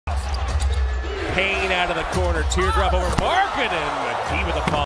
Javale McGee Dunks Over Cameron Payne (Rattata)
McGee dunks over his own teammate Cam Payne, who looks just like Rattata from Pokémon. Cam Payne in pain 🤕  with the losing campaign